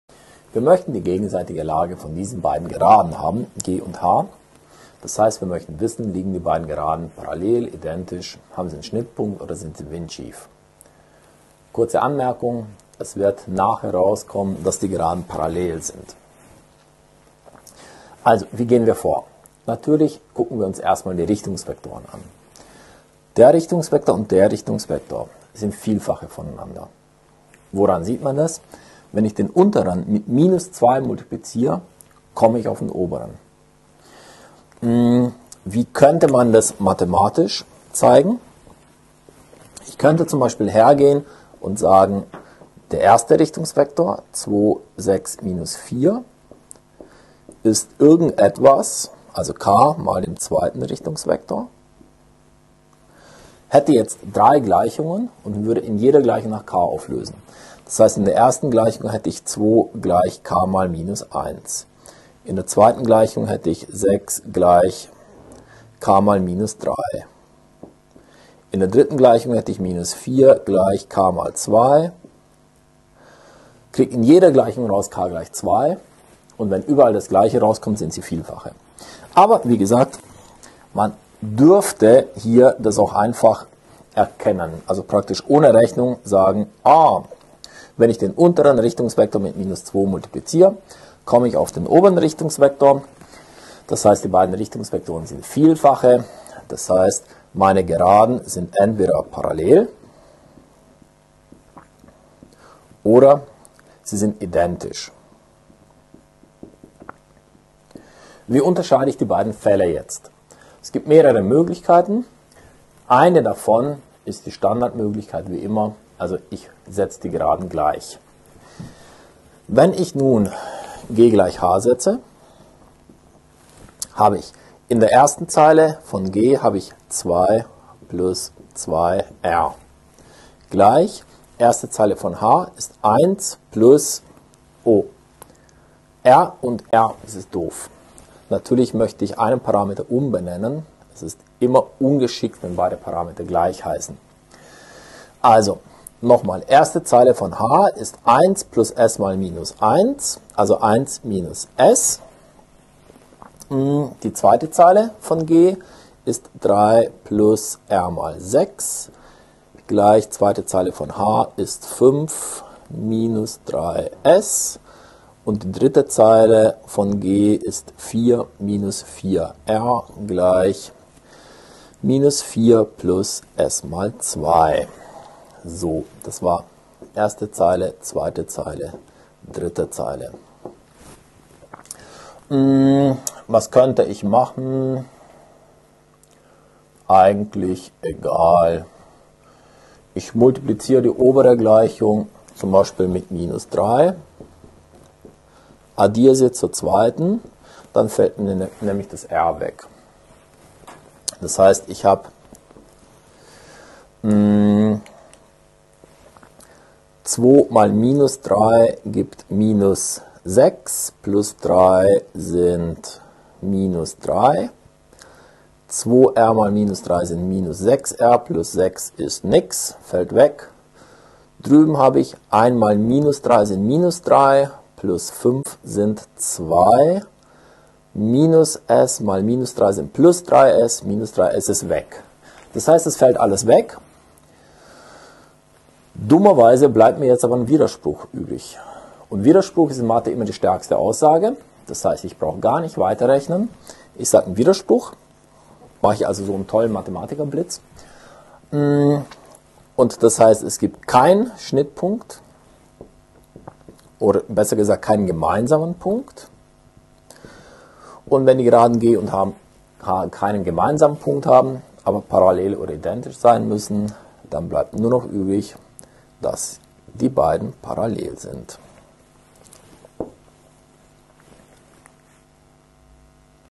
Schnittpunkt zweier Geraden berechnen, Beispiel 3 | V.02.01
Zwei Geraden können auf vier Lagen zu einander liegen: wenn die Richtungsvektoren beider Geraden Vielfache voneinander sind, sind die Geraden parallel oder identisch. Sind die Richtungsvektoren keine Vielfache voneinander, so liegen die Geraden windschief oder sie haben einen Schnittpunkt. Vorgehensweise: Man betrachtet die Richtungsvektoren beider Geraden und danach setzt man die Geraden gleich.